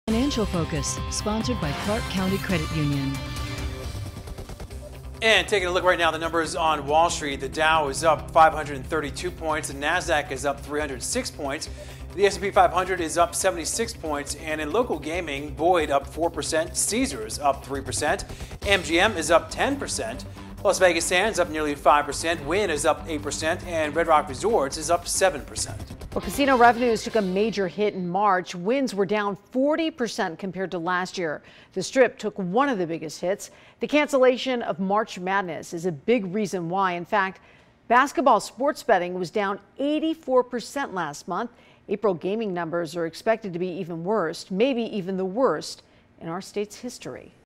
Financial Focus for April 29, 2020
In today's Financial Focus, we have a check of the stock market and the stocks with ties to Las Vegas.